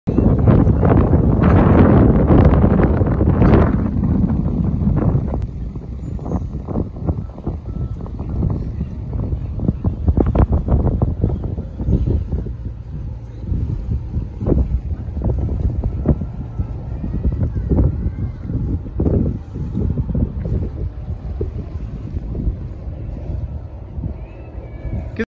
Shenzhen Bay: Beautiful Sea View
This video provides you a short view of Shenzhen Bay. Located in Guangdong province of China.Enjoy the video.